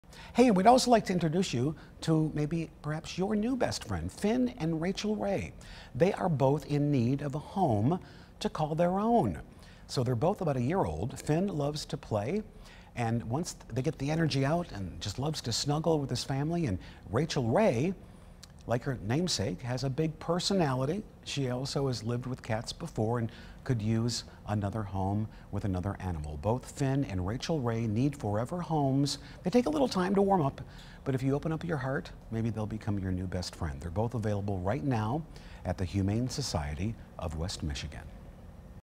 Adopt-A-Pet: Meet Finn and Rachael Ray!
Finn is a 1-year-old medium sized mixed breed who loves to play! His hobbies include the "zoomies," frolicking and jumping. But once all that energy is out, he loves to snuggle with his family!